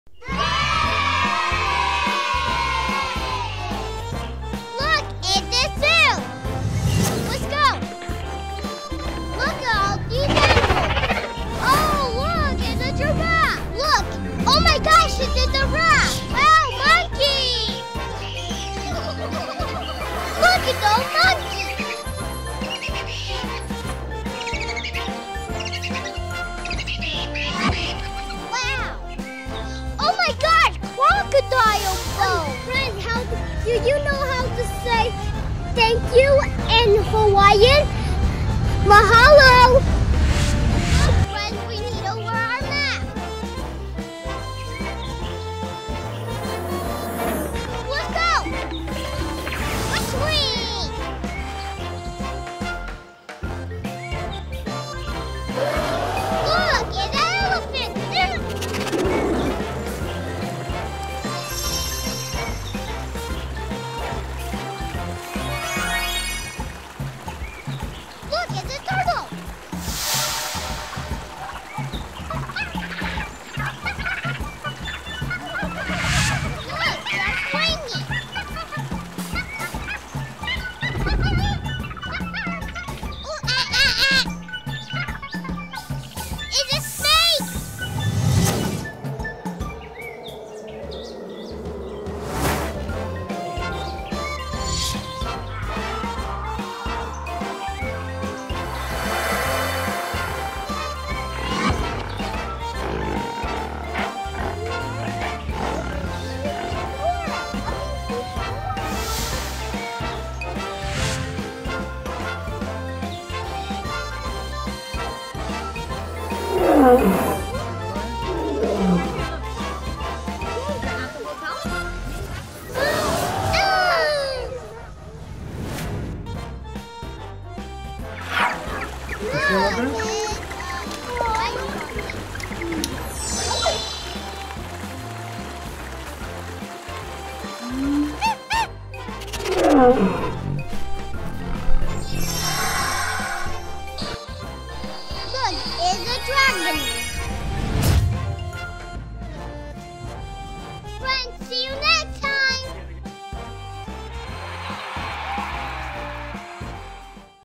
Max at the Honolulu Zoo
Visit the Honolulu Zoo with Max as we see the Monkeys, Elephants, Alligators and more. There is lots to see at the Honolulu Zoo !